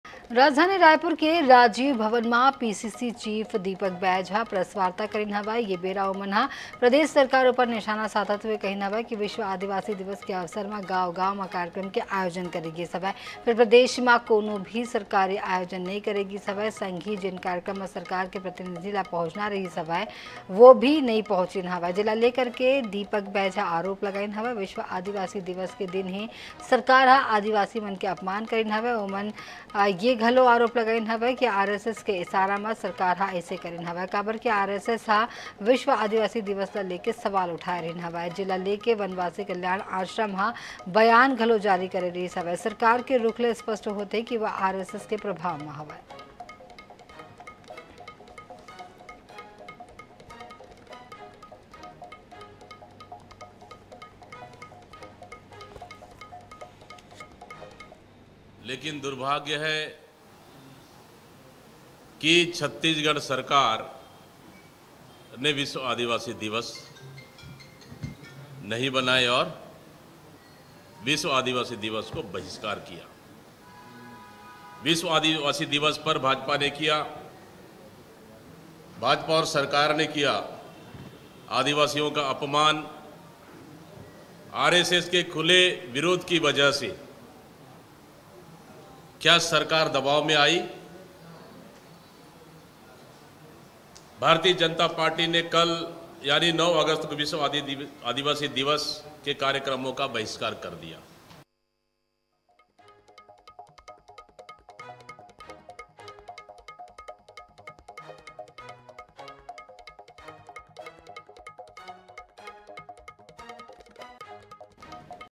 Deepak Baij प्रदेश सरकार उपर लगाइस गंभीर आरोप, कहिस - सरकार ह आदिवासी मन के करिस अपमान
INH, Deepak Baij  प्रदेश सरकार उपर लगाइस गंभीर आरोप, कहिस - सरकार ह आदिवासी मन के करिस अपमान

Incoming Search Terms: